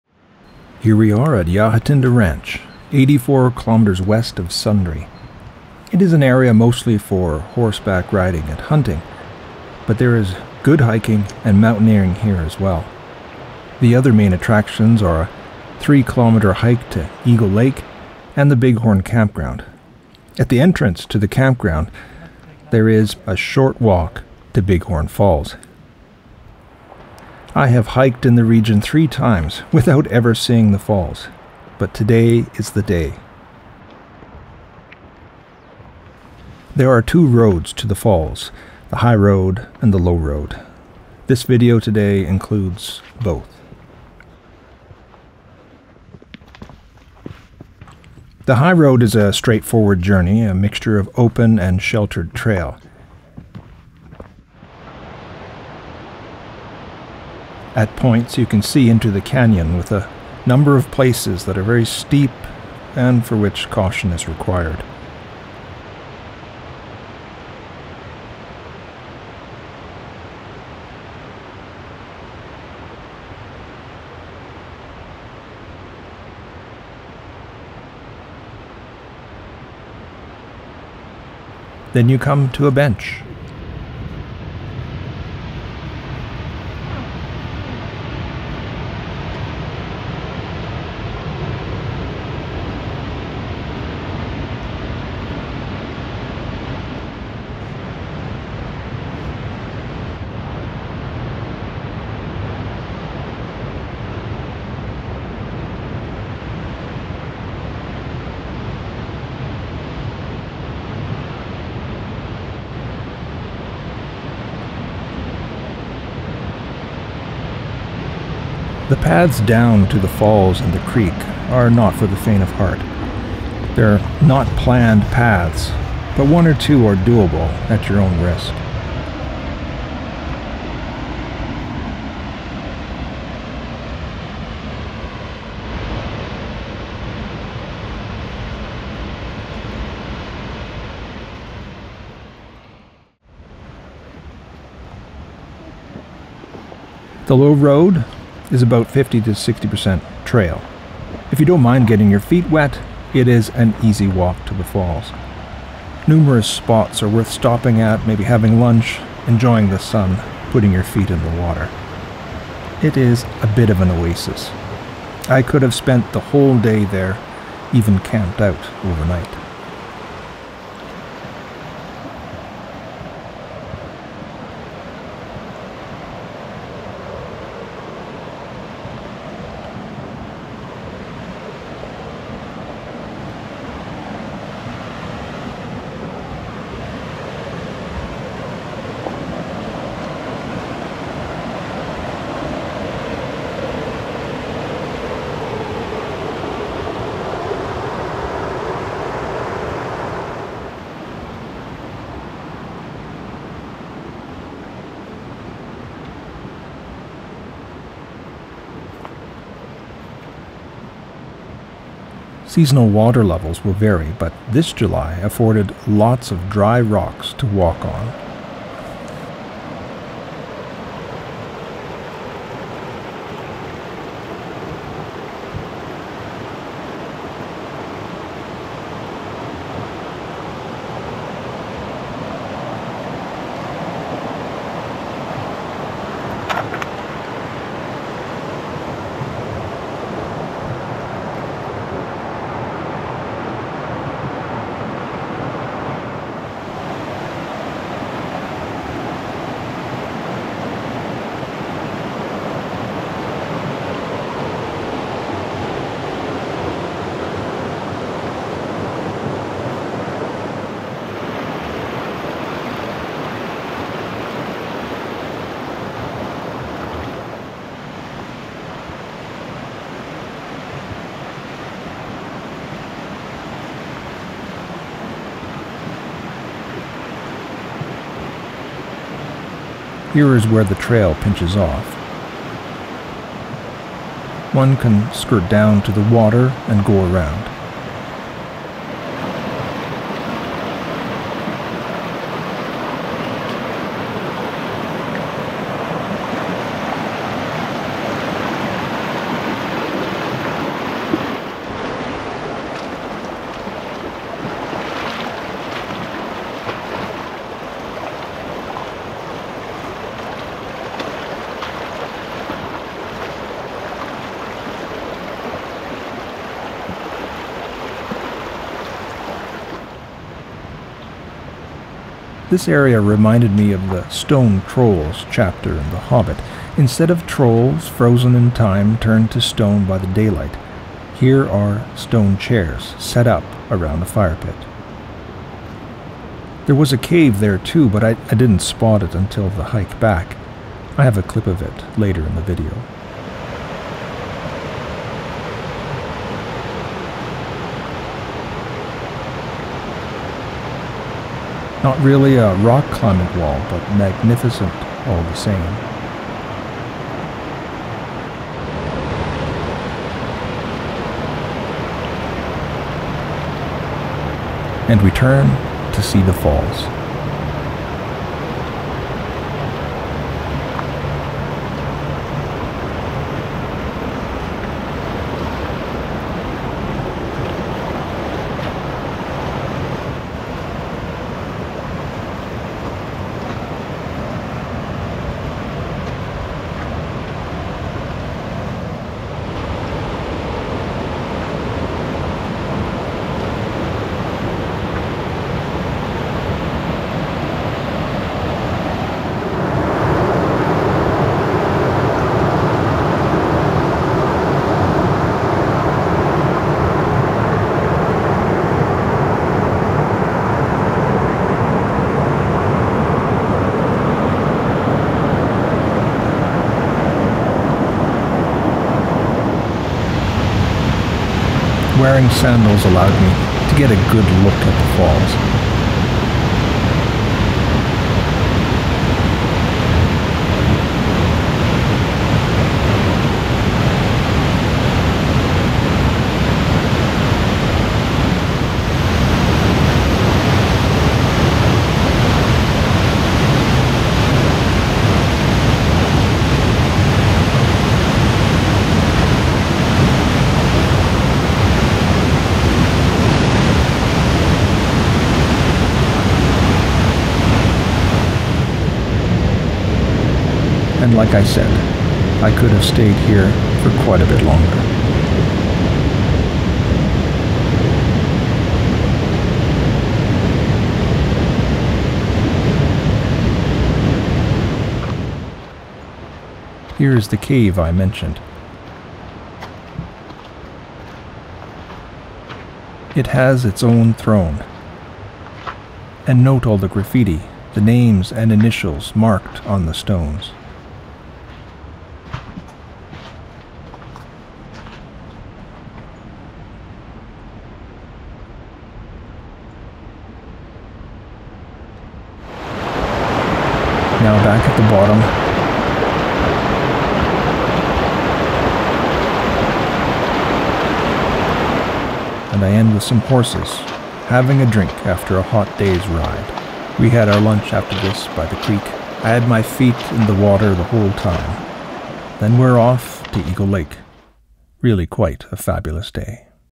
Hike to Bighorn Falls - July 8 2024
The family and I went for a hike to Bighorn Falls (and then Eagle Lake). This video is of the two paths one can take to view the Falls. One is the high road, which is the steepest, but easiest. You can then look at the top of the falls. The second is the low road. This is the path at water level. Well, there is a path for only about half of it. The rest is along side the creek. Sometimes it is hard to keep your feet wet. If you have sandals, then it is a breeze. Highly recommended.

84km West of Sundre, with gravel roads for about half of that

0:00 - Ya Ha Tinda
0:22 - upper trail
2:22 - creek trail
5:23 - stone chairs
6:00 - the Falls
7:57 - cave